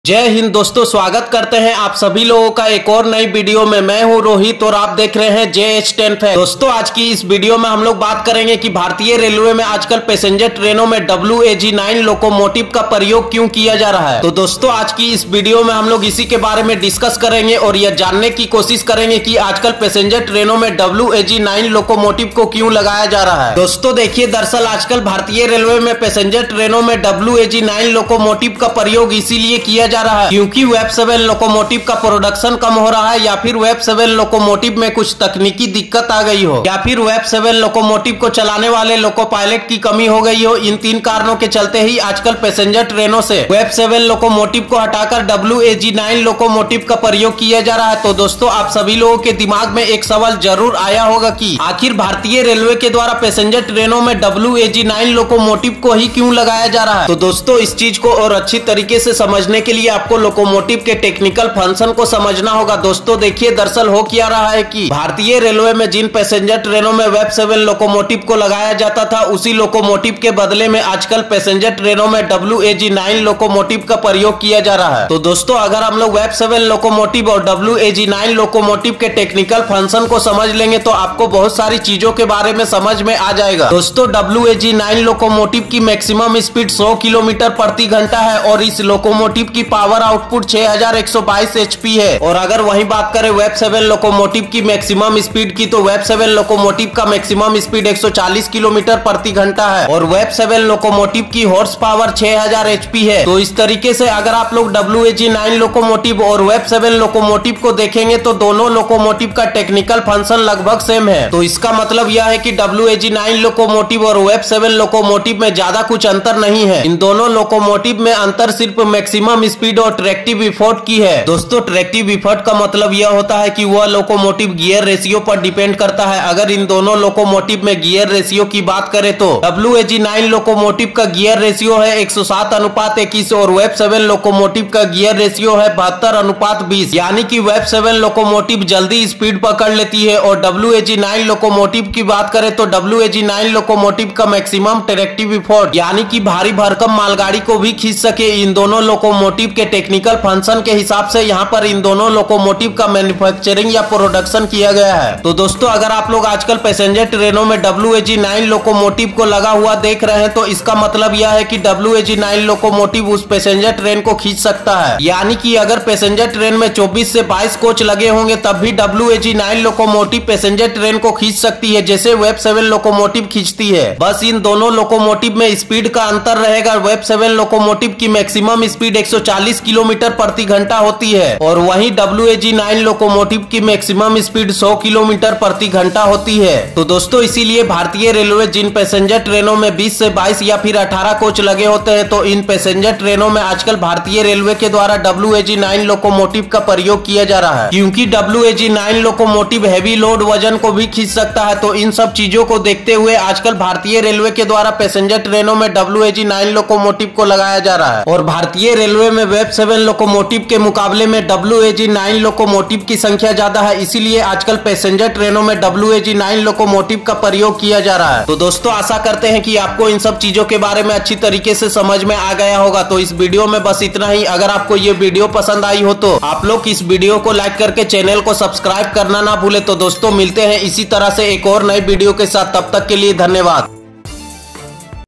WAG-9 Locomotive Passenger ट्रेन में क्यों लगाए जा रहे हैं | Indian Railways
Doston is video me ye bataya gaya hai ki aajkal bhartiye railway me passenger traino me WAG-9 Locomotive ko kiyu lagaya ja raha hai to doston iske baare me janne ke aaplog is video ko pura jarur dekhiyega